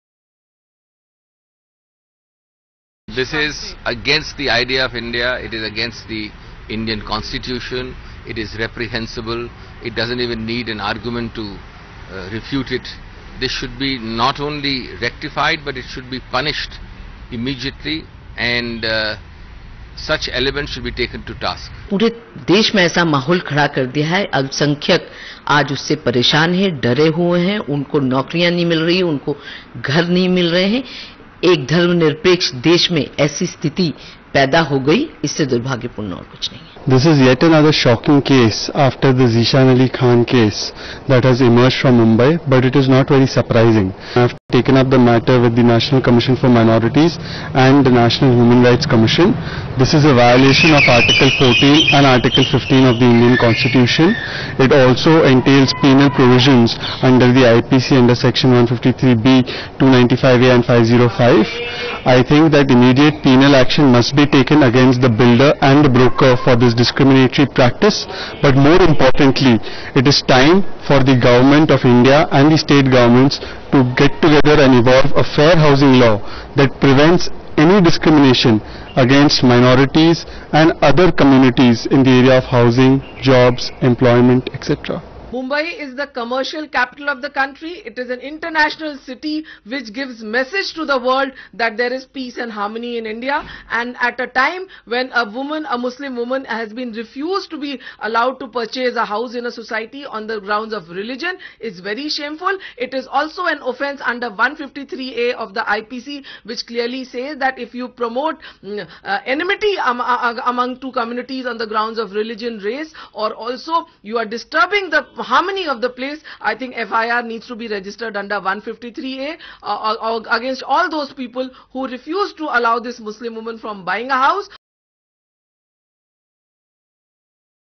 Politicos & activists react after Mumbai girl was denied accommodation for being Muslim
New Delhi/ Mumbai, May 27 (ANI): Political leaders as well as social activists on Wednesday react after a girl in Mumbai was denied accommodation for being Muslim. Congress leader Abhishek Manu Singhvi said that such discrimination is totally against the very idea of India. AICC Spokesperson Shobha Oza blamed the present government for promoting religious discrimination in the society. Social Activist Shehzad Poonawalla said that such incidents are in violation of the Indian constitution and demanded immediate action against the builder and the broker of the building. Senior lawyer Abha Singh said that this is a serious issue and a FIR should be filed in the same.